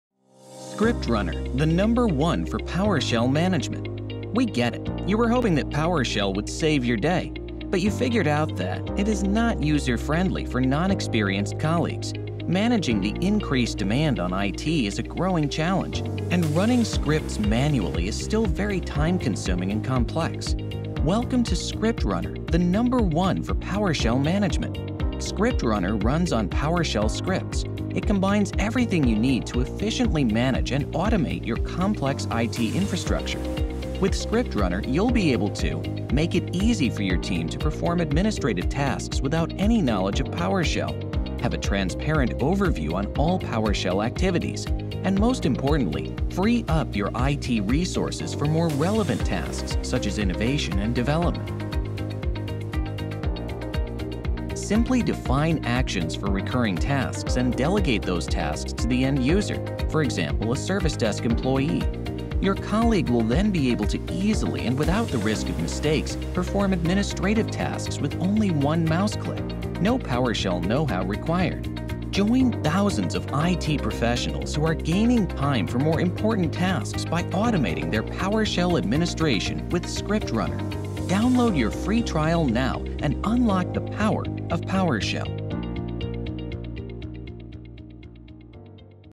ScriptRunner: The #1 for PowerShell Management
Find out what makes ScriptRunner the #1 platform for PowerShell Management and what it can do for you and your team in this video!

Centralizing, standardizing, automating, delegating, monitoring and controlling routine tasks frees up resources in IT operations. Administrators and DevOps teams can use and customize included script libraries or develop their own scripts.

ScriptRunner allows you to securely delegate administrative tasks to users without PowerShell knowledge or appropriate rights.

ScriptRunner is used worldwide by IT teams of all sizes and industries.